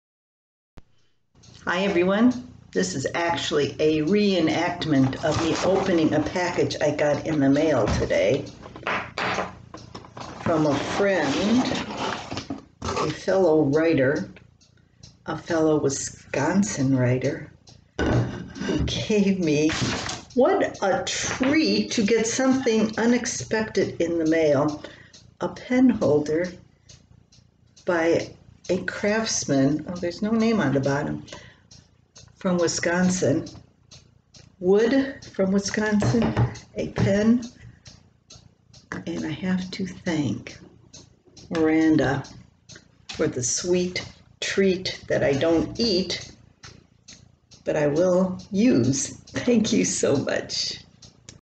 A surprise in the mail
Mail delivery out in the country is important. Yes, the USPS delivers bills but they also bring surprises like the one that came here.